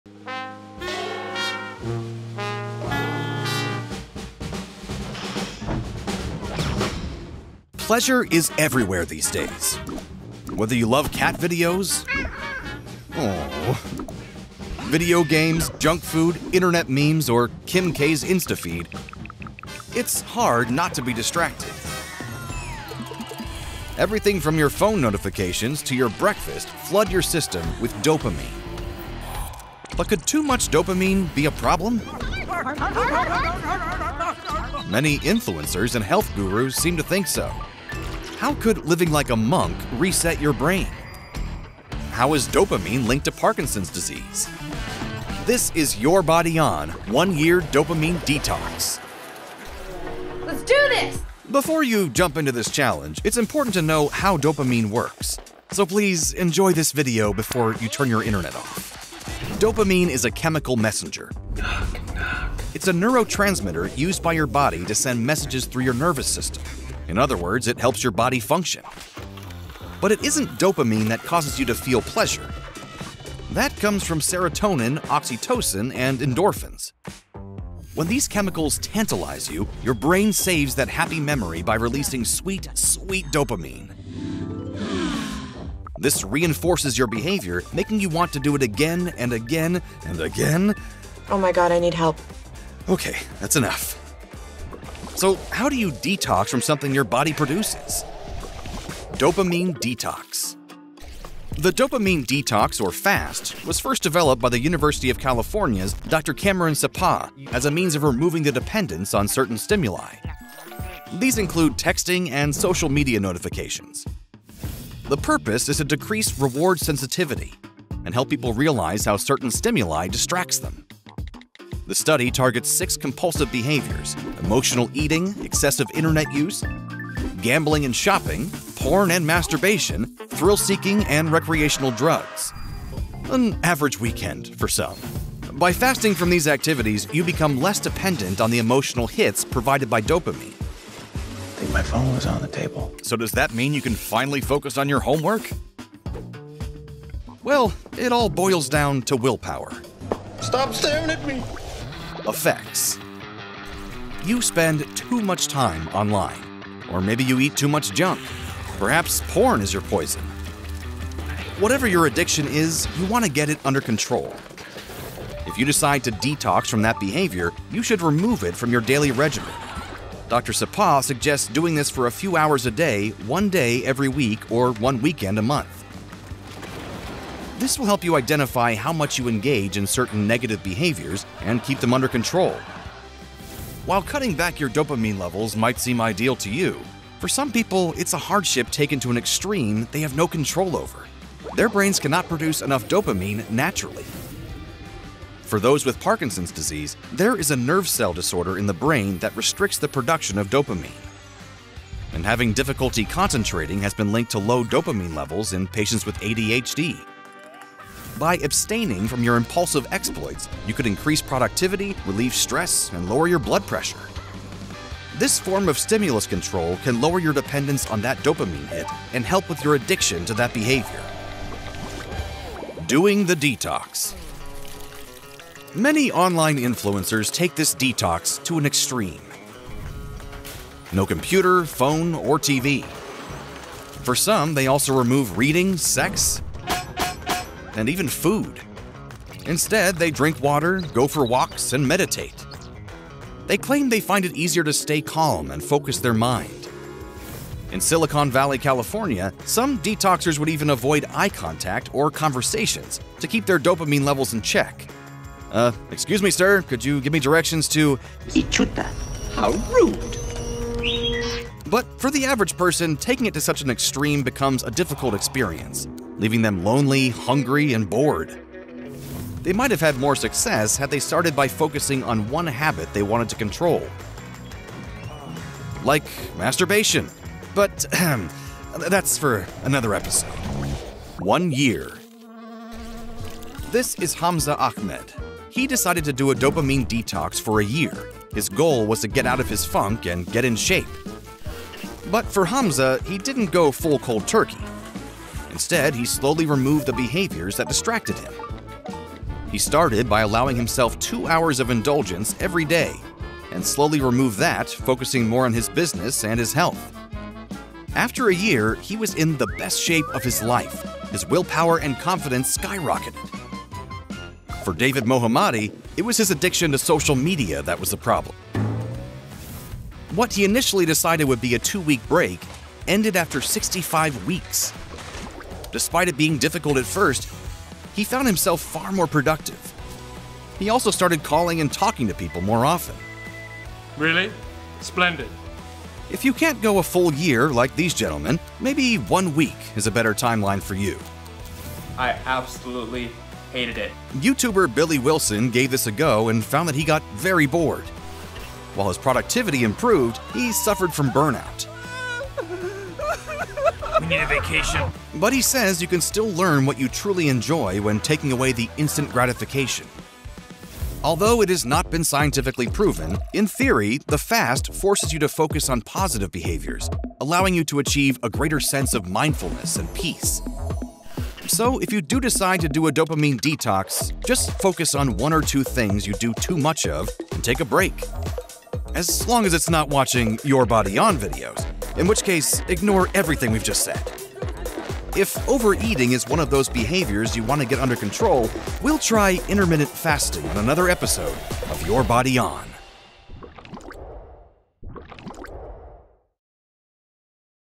How Can You Detox from Dopamine?
Pleasure is everywhere these days. Whether you love cat videos, video games, junk food, internet memes or Kim K's Insta feed, it's hard not to be distracted. Everything from your phone notifications to your breakfast flood your system with dopamine. But could too much dopamine be a problem? Many influencers and health gurus seem to think so. How could living like a monk reset your brain? How is dopamine linked to Parkinson's disease?

00:00 How Can You Detox from Dopamine?
00:52 How your brain works
01:40 Dopamine detox
02:33 Effects
03:43 Doing the detox
04:42 One year